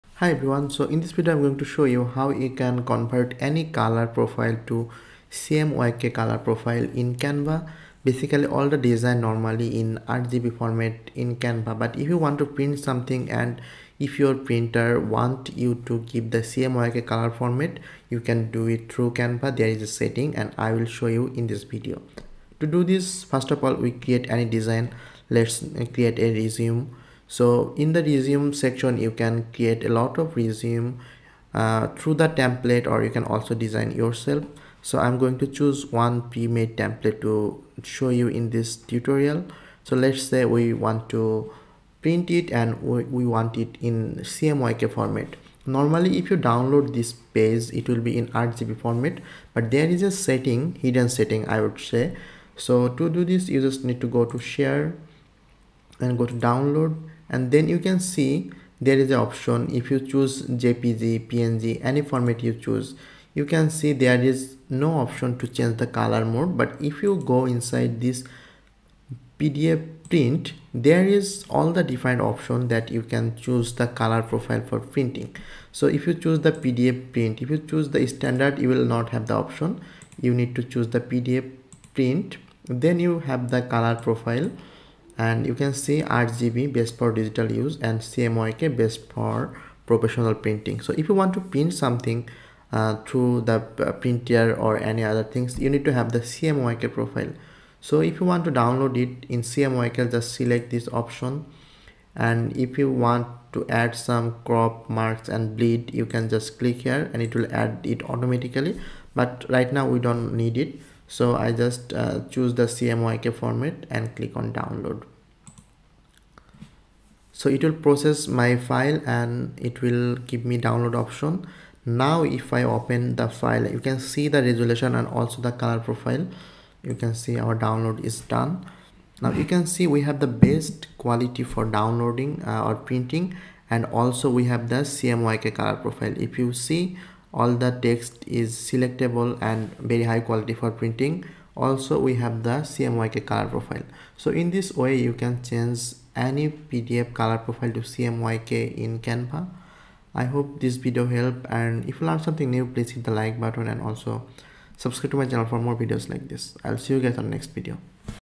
How to convert RGB to CMYK in Canva | EXPORT CMYK in Canva PDF
Dive into the world of color transformation with this quick Canva tutorial. Discover the nuances between RGB and CMYK, master the conversion process, and efficiently export your designs in CMYK PDF format. Elevate your design game in just a few simple steps!

🔍 Timestamps:

0:00 Introduction
0:15 RGB vs. CMYK
0:45 Converting RGB to CMYK in Canva
1:30 Exporting as CMYK PDF
2:15 Tips for Color Accuracy
2:30 Final Thoughts